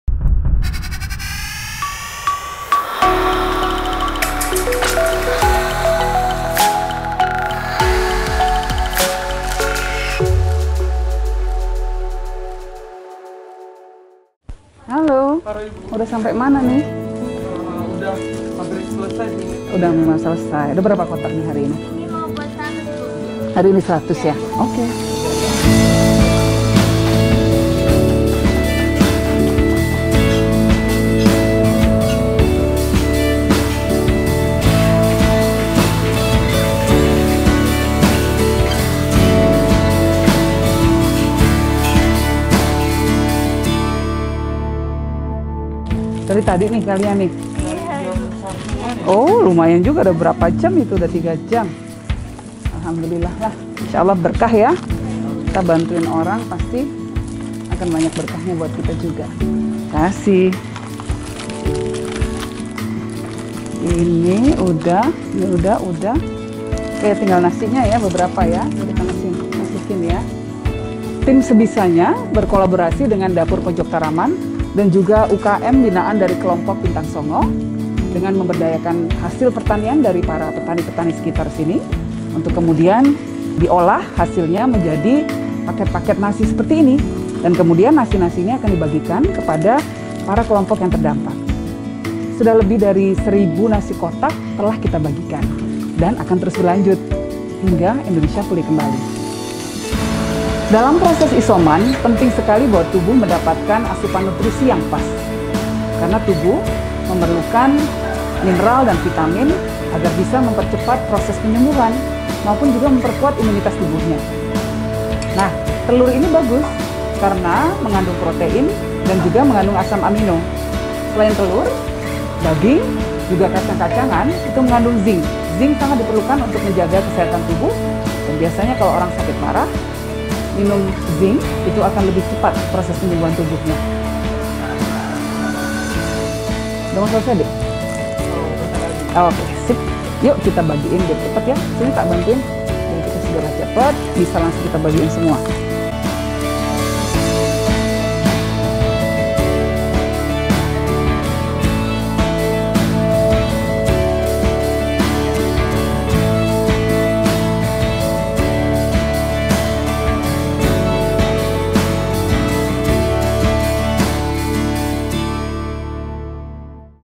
FIGUR | Putri Gus Dur Yenny Wahid Rela Masak untuk Isoman
Sebagai wujud kepedulian terhadap warga yang menjalani isolasi mandiri (isoman), Yenny Wahid pun ikut turun ke dapur membantu relawan Sebisanya untuk menyiapkan makanan untuk mereka. Kegiatan memasak itu dilakukan di Peace Village, Sinduharjo, Ngaglik, Sleman yang menjadi salah satu posko Sebisanya, Senin (2/8/2021).

Kasus Covid-19 di Indonesia masih terus bertambah. Sejak Maret 2020 hingga 2 Agustus 2021, kasus Covid-19 di negeri ini mencapai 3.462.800 orang.

Angka kematian pada 27 Juli 2021, bahkan tembus rekor mencapai angka 2.000 orang per hari. LaporCovid-19 mencatat sebanyak 2.833 total kematian warga saat menjalani isolasi mandiri dan di luar rumah sakit. Hal ini berdasarkan pembaruan data per 30 Juli 2021.

Yenny Wahid mengatakan dalam proses isoman, sangat penting untuk memenuhi kebutuhan nutrisi tubuh agar stamina dan imunitas tetap terjaga. "Penting sekali bahwa tubuh mendapatkan asupan nutrisi yang pas karena tubuh memerlukan mineral dan vitamin agar bisa mempercepat proses penyembuhan maupun juga memperkuat imunitas tubuhnya," kata dia.

Ia menyebutkan pemilihan bahan makanan yang tepat harus diperhatikan untuk menunjang kesehatan. Misalkan telur yang dinilai bagus karena mengandung protein dan asam amino.

"Daging juga kacang-kacangan itu mengandung zink. Zink sangat diperlukan untuk menjaga kesehatan tubuh dan biasanya kalau orang sakit parah minum zink itu akan lebih cepat proses penyembuhan tubuhnya," papar dia.

Putri Gus Dur ini mengatakan, dalam kegiatan berbagi ini, tim Sebisanya berkolaborasi dengan dapur Pojok Taraman dan juga UKM binaan dari kelompok Bintang Songo dengan memberdayakan hasil pertanian dari para petani-petani sekitar.

"Hasil pertanian itu kemudian dibuat, diolah, dan hasilnya menjadi paket-paket nasi dan kemudian nasi-nasinya akan dibagikan kepada para kelompok yang terdampak," ujar dia.

Tim Sebisanya sudah lebih dari sepekan melakukan aksi bagi nasi kotak dan bahan makanan bagi warga yang sedang menjalani isoman serta yang terdampak pandemi. Hingga 30 Juli 2021, jumlah bantuan yang telah dibagikan sebanyak 3.200 buah nasi kotak dan 90 paket bahan makanan. Kegiatan ini akan terus berlanjut hingga Indonesia pulih kembali.